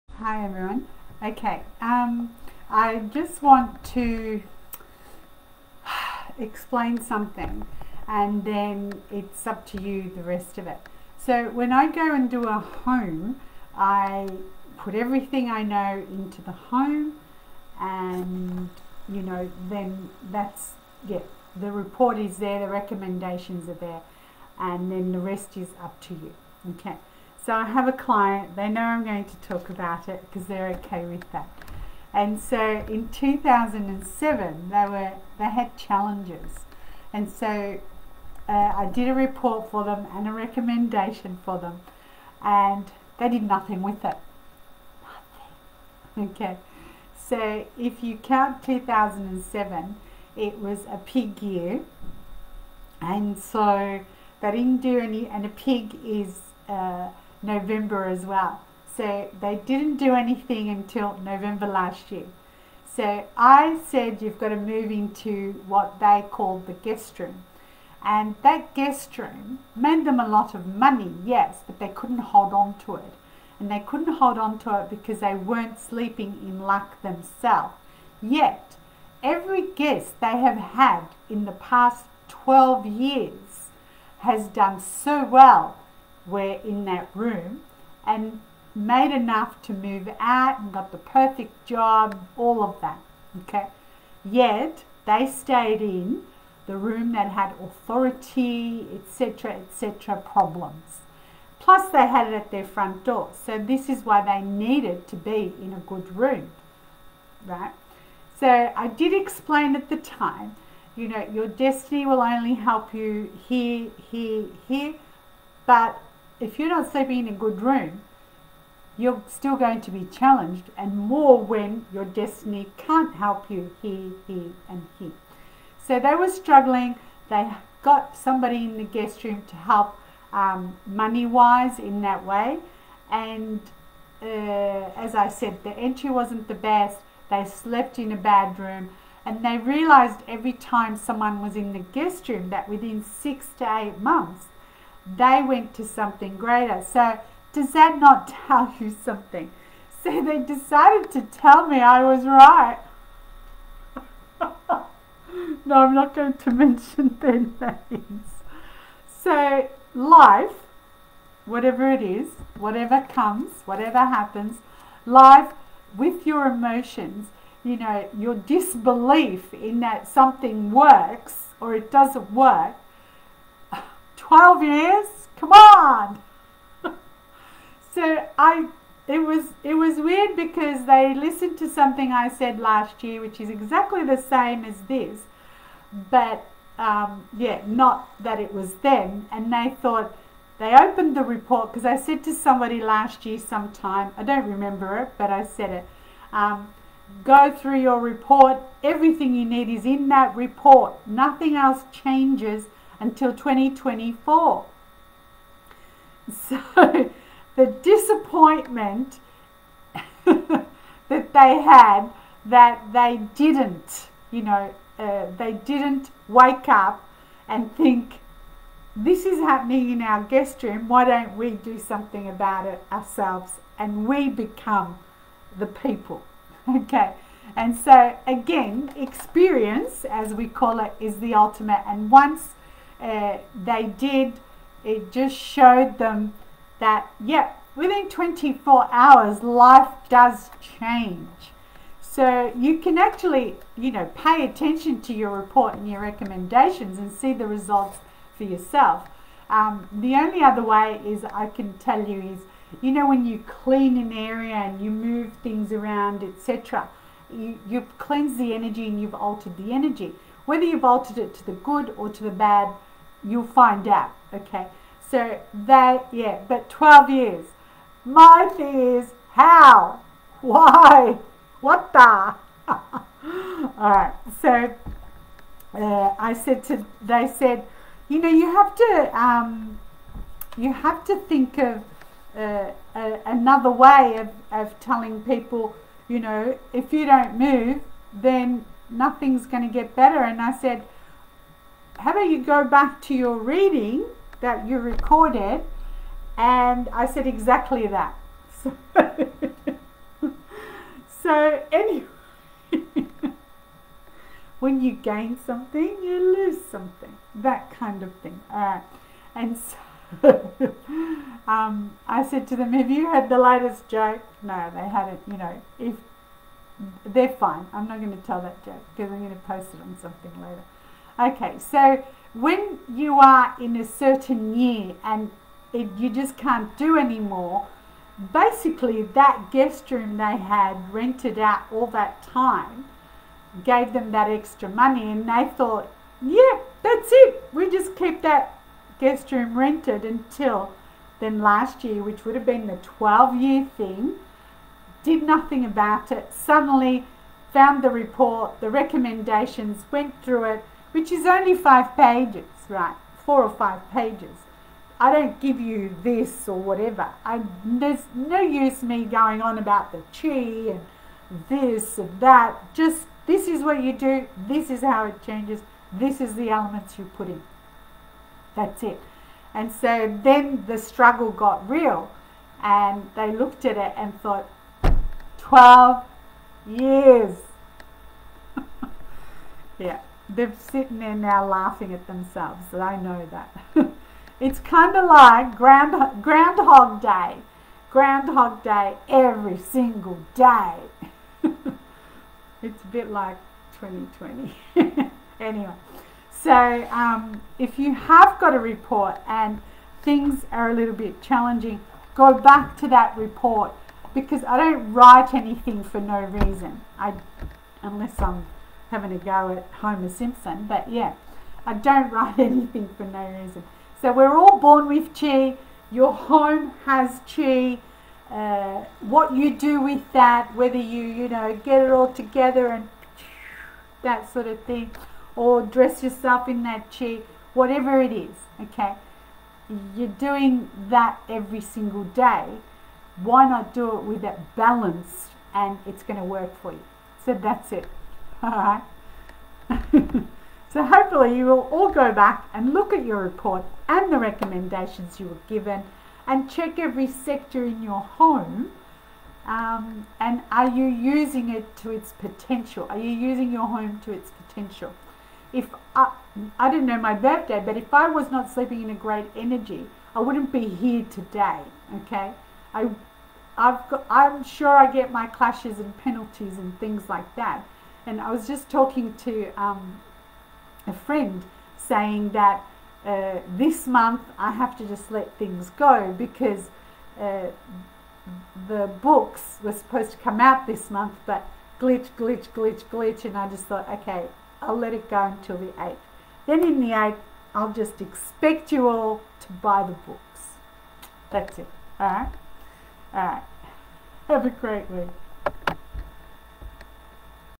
Feng Shui Client waited 12 years.
A client had to let me know that what I wrote in the report and recommended to their home was true and works. Baaahaaa.
Tell me about it!
Sometimes you have to hit rock bottom more than once to realise what you are doing is wrong. Ground-hog day for 12 years? again Whhhhyyyy?
With blessings